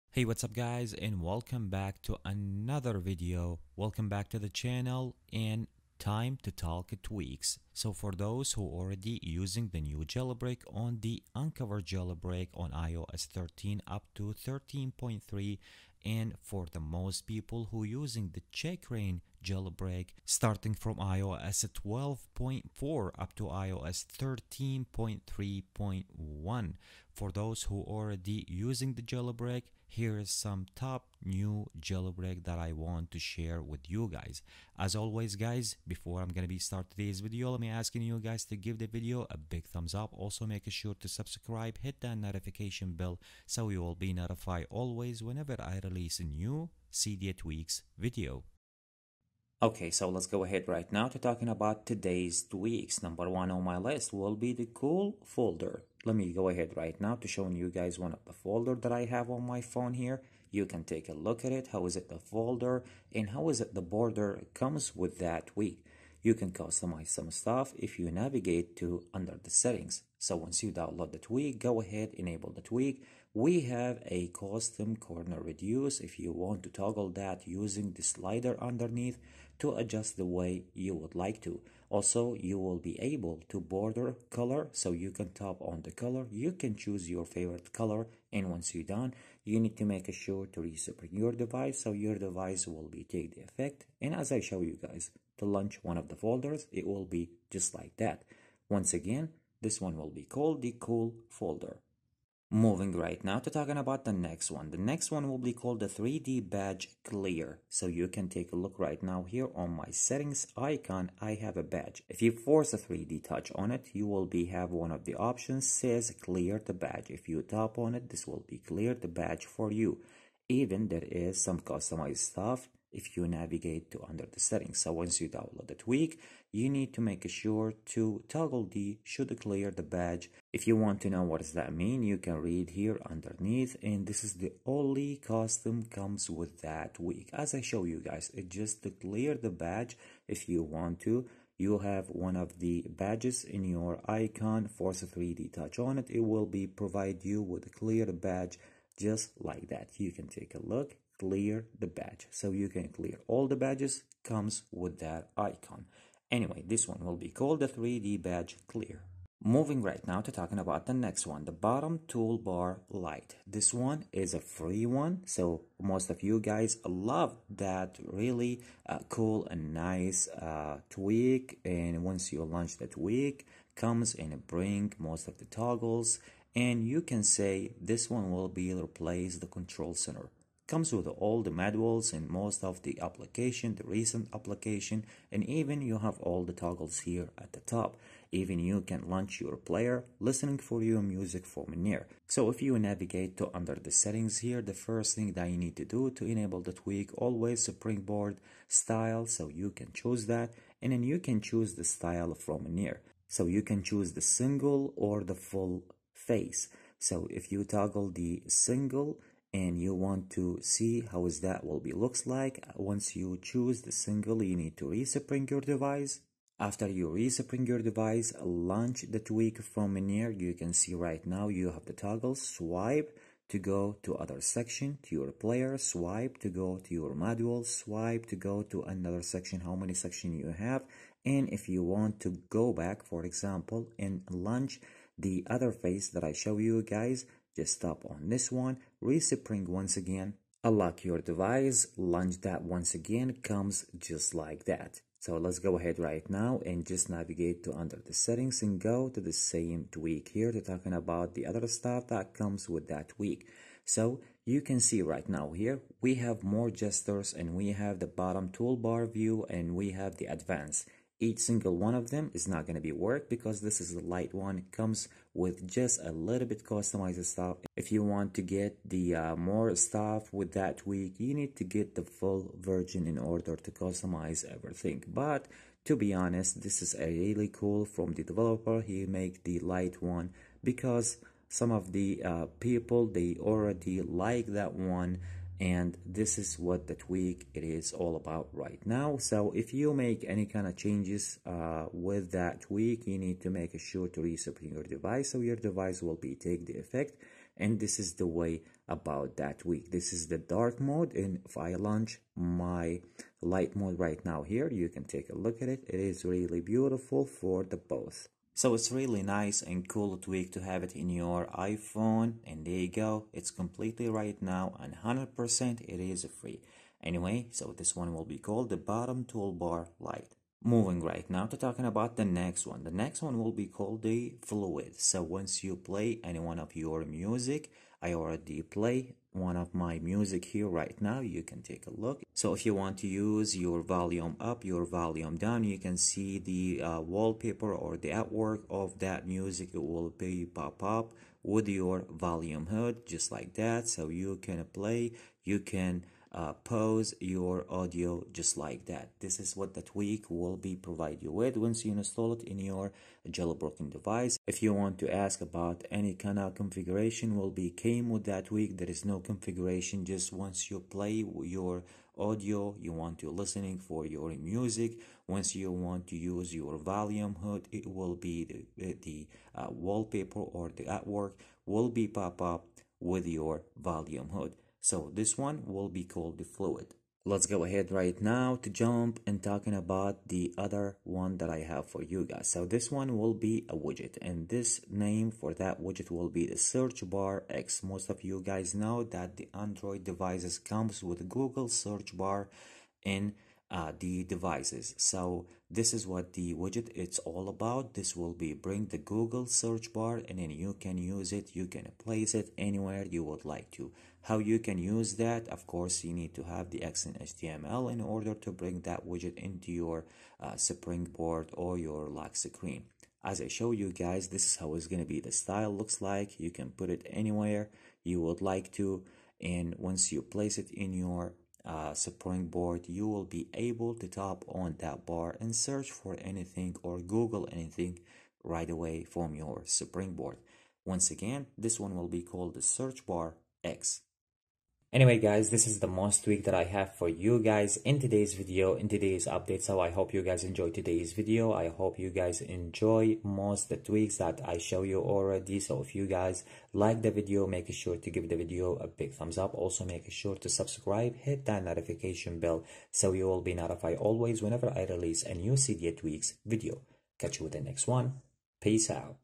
Top New A12 - A13 Jailbreak Tweaks for iOS 13 / 13.3! (Unc0ver + Checkra1n)
iOS13 Perfect Jailbroken iPhone: Top 13 BEST iOS 13 Jailbreak Tweaks for Cydia. Customize your iPhone on iOS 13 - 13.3 and get NEW features on the Unc0ver / Checkra1n Jailbreak

BottomToolBar Lite
Fluide
Search BarX

Theme :  Lotus